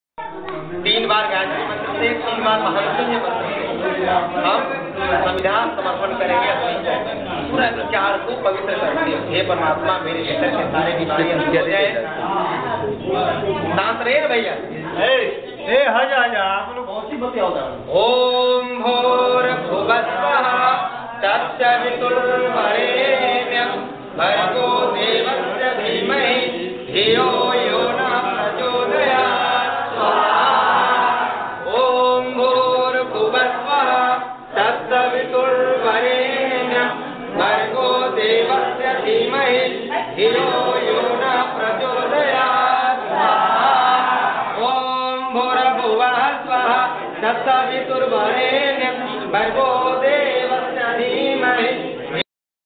Hawan Yajya|Yogi Dr. Brij Mohan |Yog Pracharak-Patanjali Yogpeeth,Haridwar In Dihri,Bhojpur,Bihar
01 State-BIHAR,District-BHOJPUR,Month-September-2016,Camp No.-01,Time-05.00AM TO 08.30AM,Yog Shivir-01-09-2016 To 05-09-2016,Venue-Vill-Dihri,Baligaon,Block-Garhani,District-Ara(Bhojpur),State-Bihar,Video Cliping  Date-05/09/2016